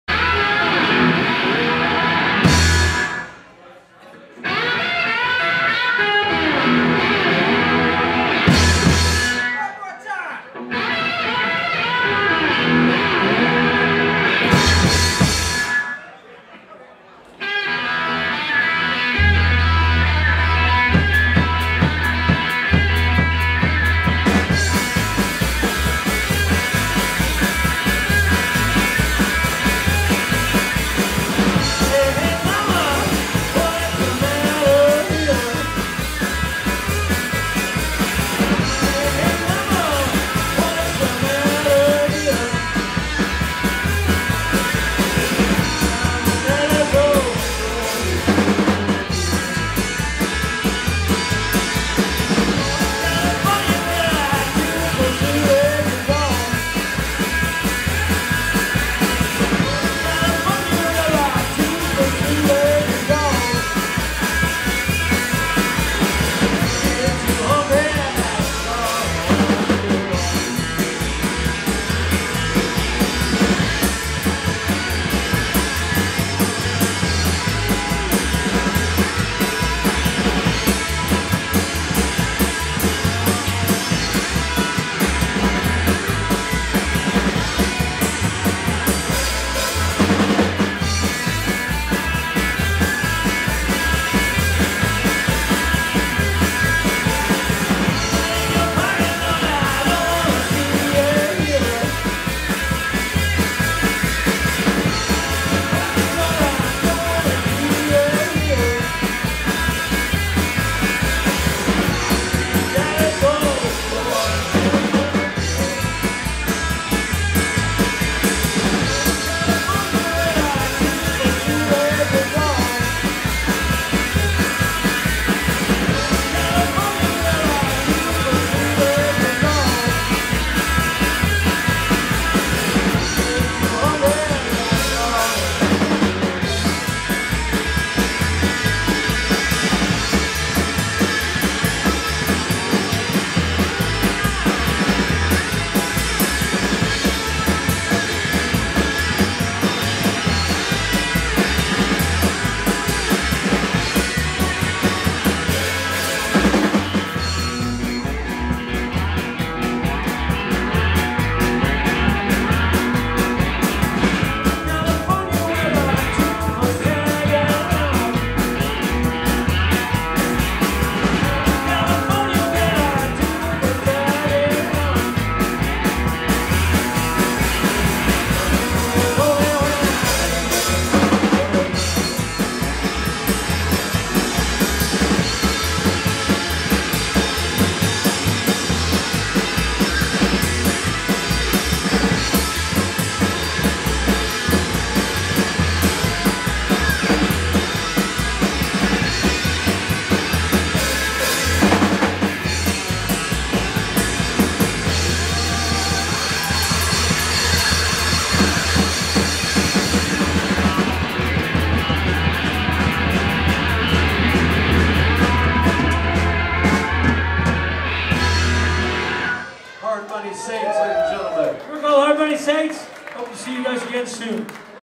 Hard Money Saints (14) May 10 2014
Hard Money Saints the 6th band to play our Everett Rock Party at The Rockin' M BBQ in Everett WA